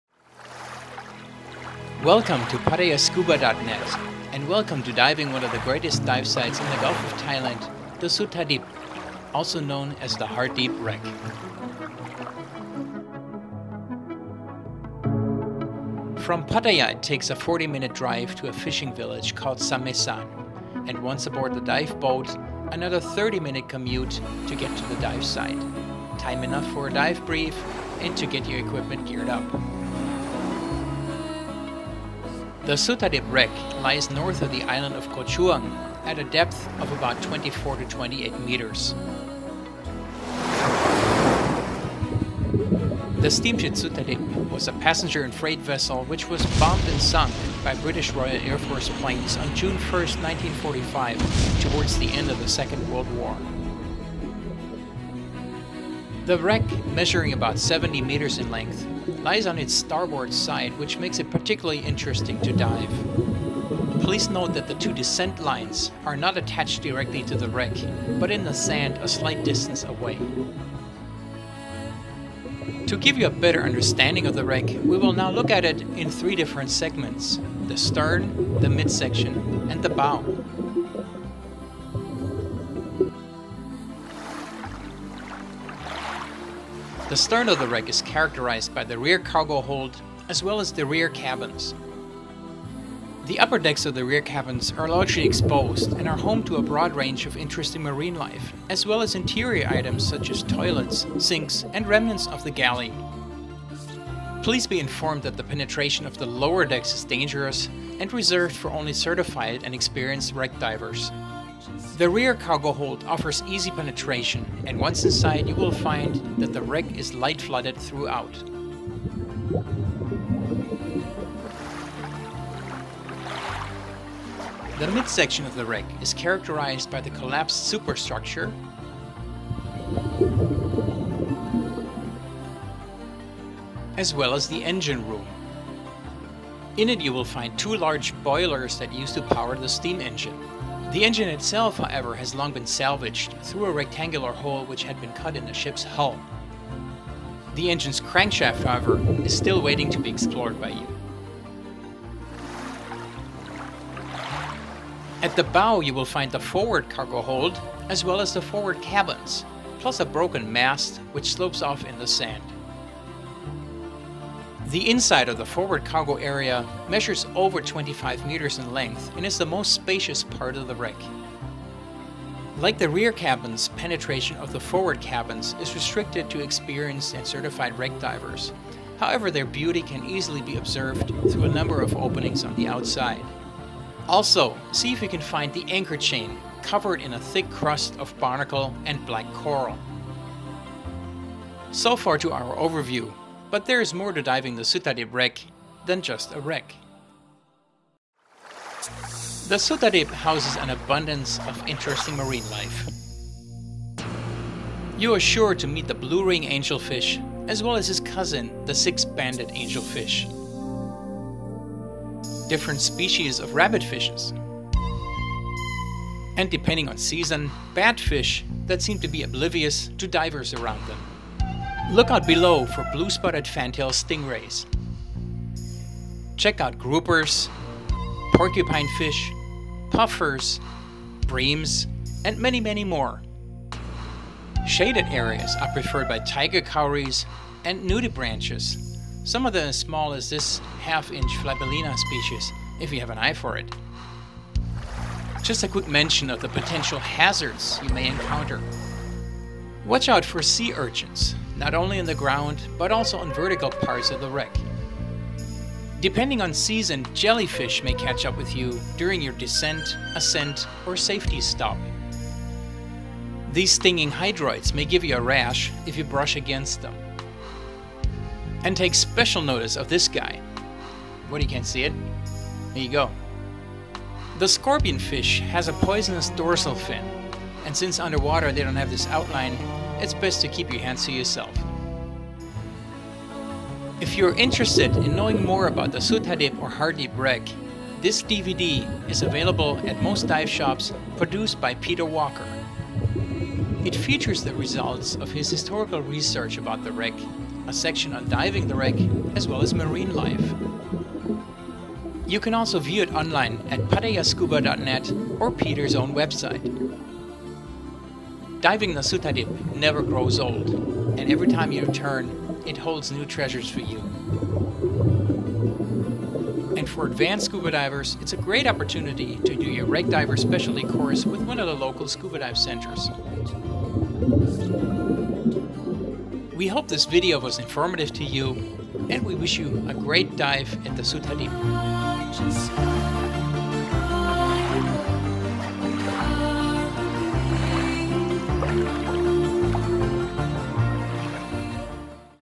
Introduction to diving the Suddhadib Wreck (Hardeep Wreck)
This is a visual slide show that serves as an introduction of the Suddhadib Wreck to first time divers. It contains background information, an overview of the wreck in 3 separate segments, common marine life, potential hazards and other information useful as a dive brief.